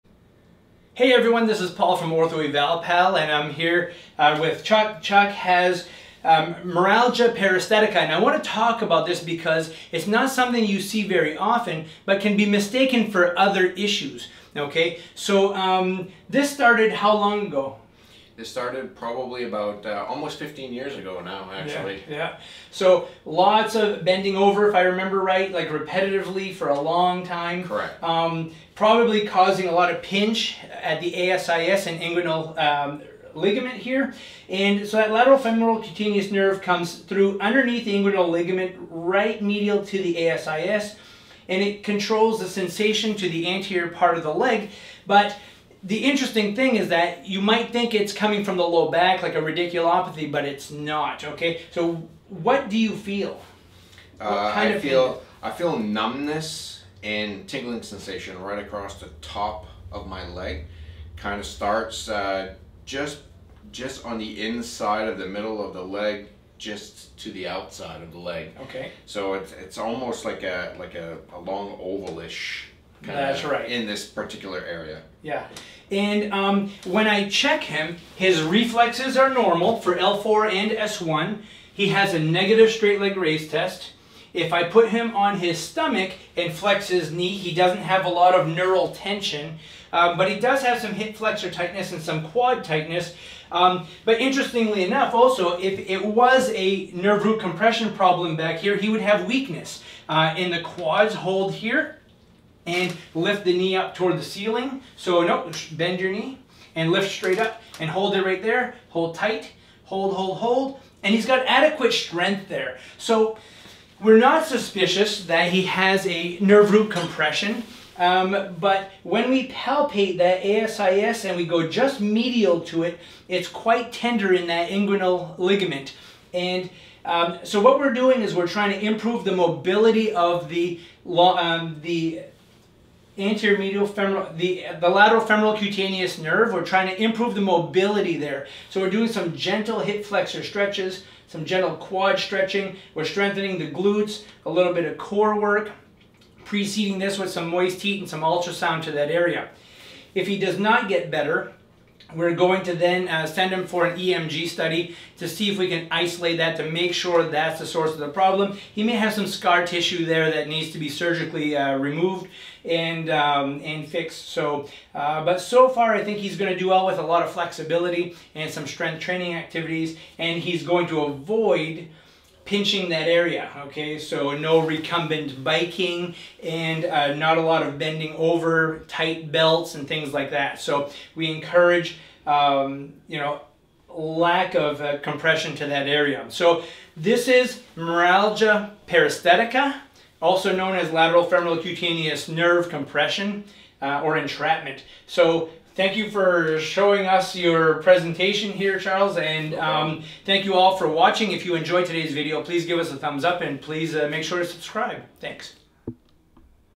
Meralgia Paresthetica (Outer thigh numbness, burning and tingling)
Here I have a patient with Meralgia Paresthetica (Lateral Femoral Cutaneous Nerve Entrapment or outer thigh numbness, burning and tingling)
I review the following:
▶The anatomical route of the Lateral Femoral Cutaneous Nerve
▶Causes of Meralgia Paresthetica
▶Signs and symptoms of Meralgia Paresthetica
▶How to manage and treat Meralgia Paresthetica and so much more!

👉MedBridge: Online CEUs - For PTs, OTs, SLPs, ATs, & RNs
👉Save $175 with your subscription NOW!!!